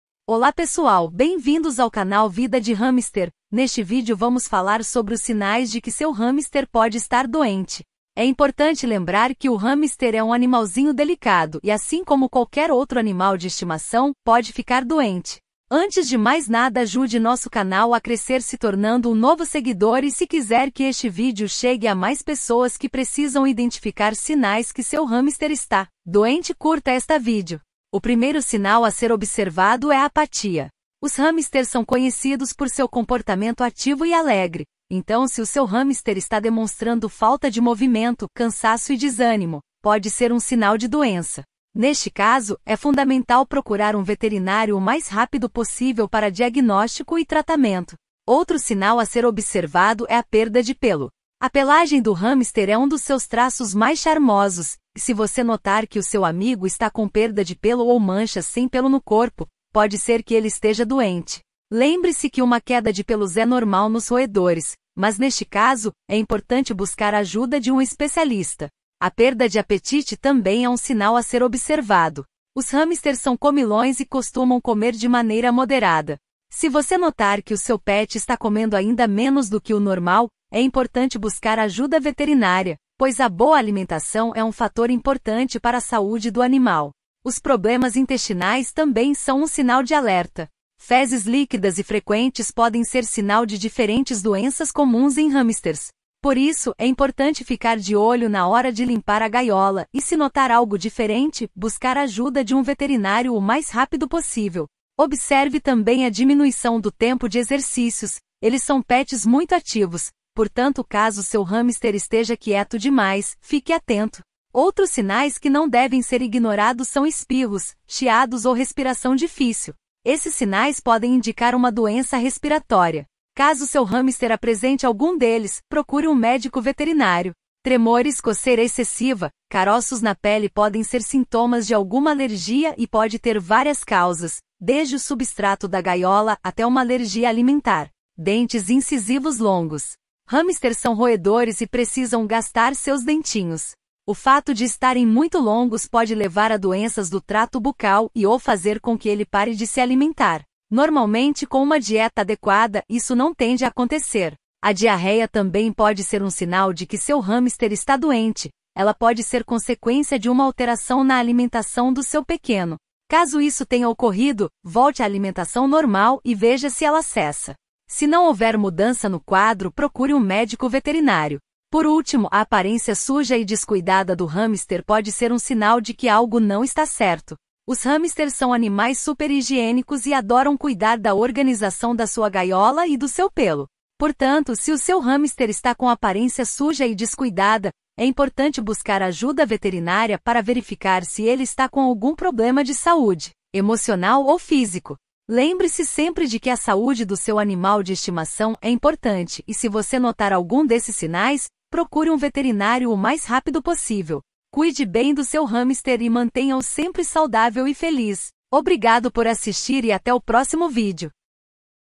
10 sinais de que o seu HAMSTER pode estar DOENTE [NÃO IGNORE O SEXTO]
Se você tem um hamster de estimação, é importante estar atento aos sinais de que algo não está bem com o seu amiguinho peludo. Neste vídeo, vamos mostrar 5 sinais que indicam que o seu hamster pode estar doente, desde apatia até problemas intestinais.

 Além disso, destacamos a importância de levar o seu pet ao veterinário assim que notar qualquer um destes sinais.

Afinal, a saúde do seu hamster é tão importante quanto a sua felicidade. Assista agora e aprenda como cuidar melhor do seu amigo roedor!

🐹 Ebook Hamster Feliz: Manual - Completo Sobre Como Cuidar de seu Hamster
🐹 Produtos para Hamster Amazon